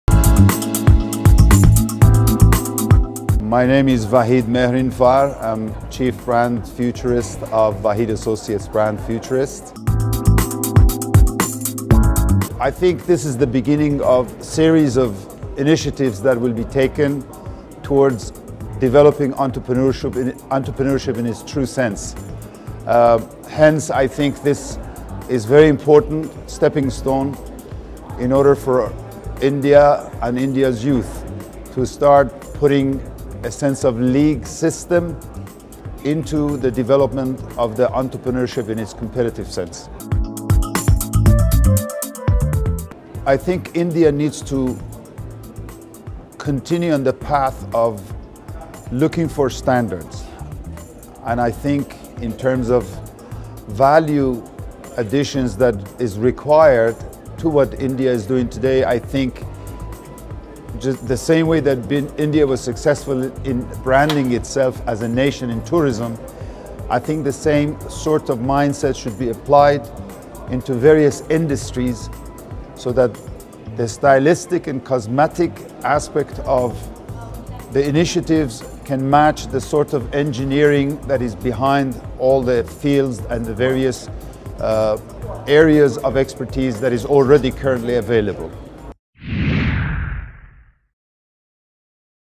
Porter Prize - CEO Talks - Vahid Mehrinfar- Chief Brand Futurist, Vahid Associates Brand Futurists
Interview with Vahid Mehrinfar- Chief Brand Futurist, Vahid Associates Brand Futurists on competitiveness landscape in India and the potential impact of the Porter Prize